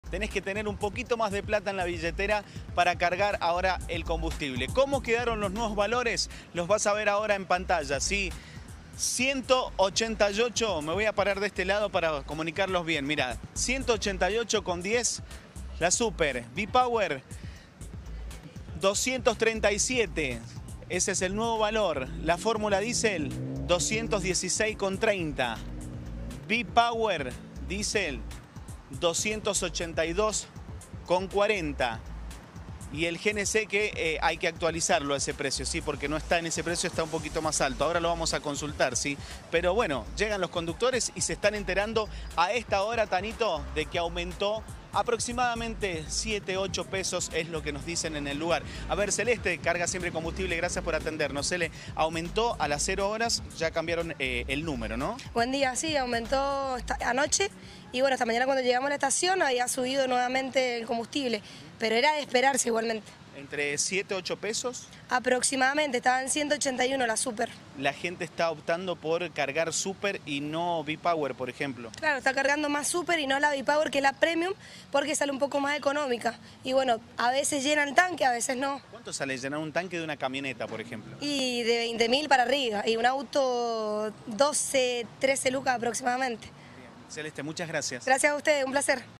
COMBUSTIBLES MÁS CAROS
La petrolera Shell informó que desde este miércoles 15 de marzo aumentó 3,8% en promedio el precio de todos sus combustibles, en un incremento que se dio a conocer horas después de que se  anunciara la inflación de febrero que correspondió al 6,6%.